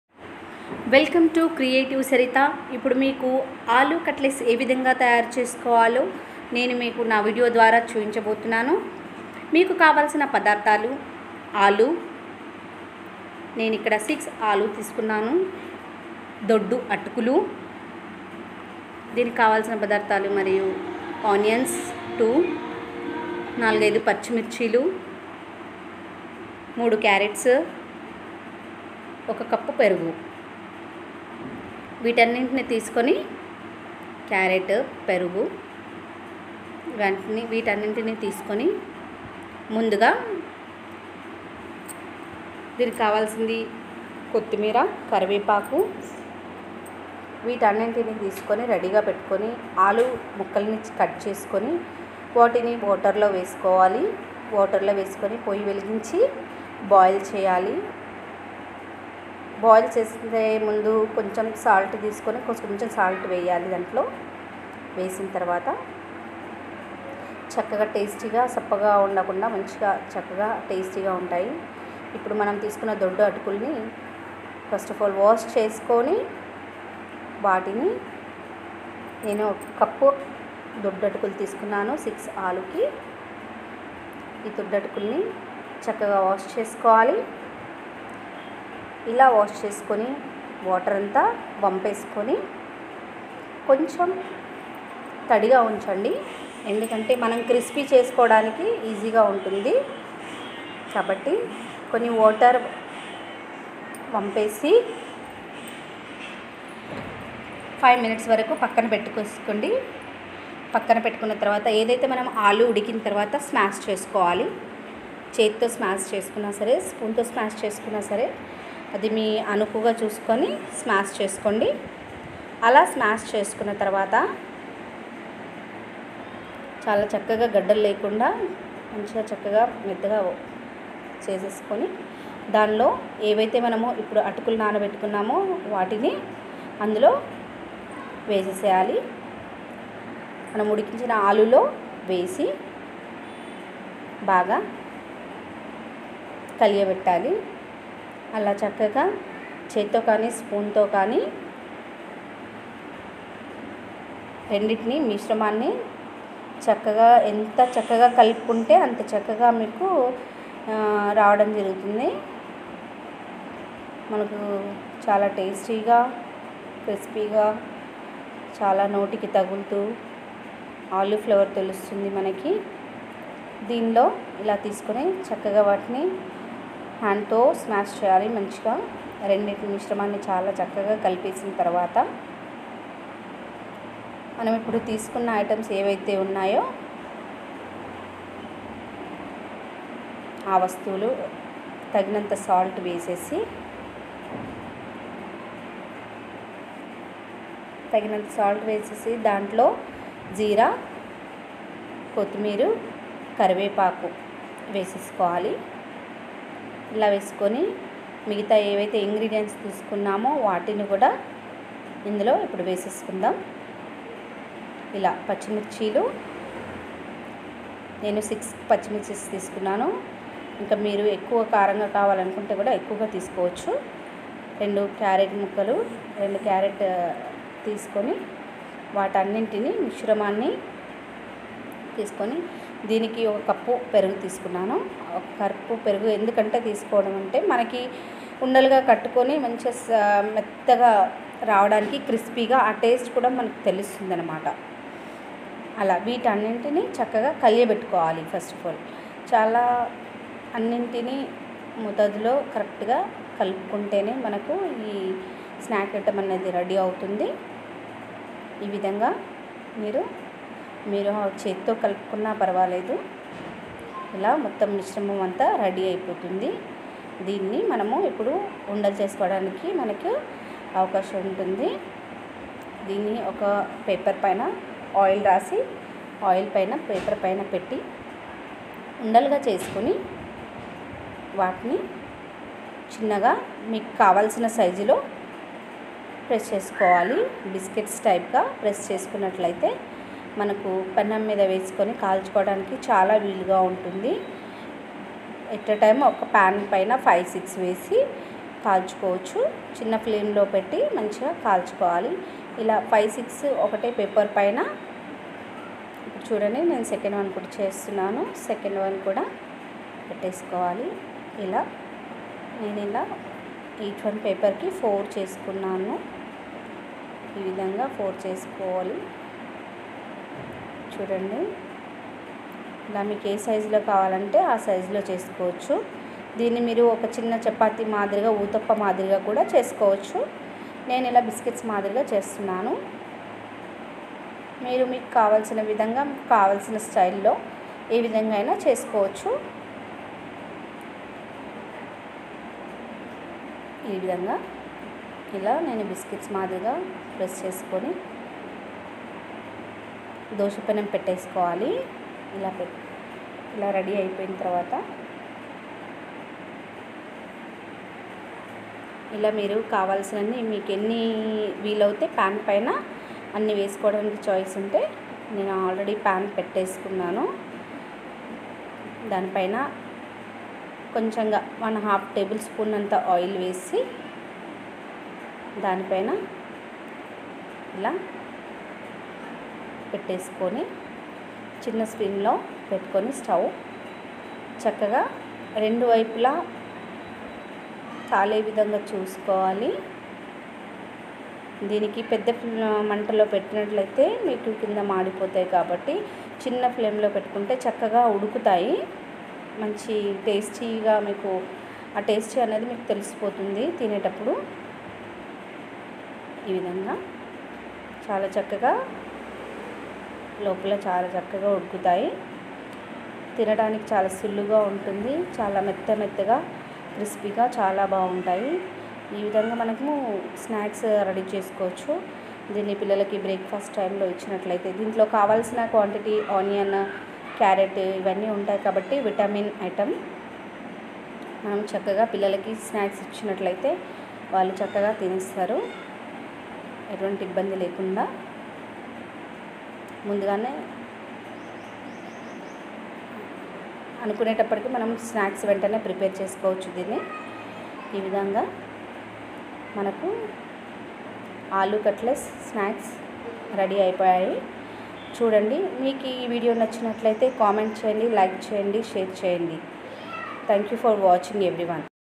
ఆలు కట్లేస్స్ స్నాక్స్ ఈ విధంగా రుచిగా .....................
`welcome to creative saritha now i will show how to prepare very crispy and tasty snack item it was very healthy and energetic food item if you want more snack item please comment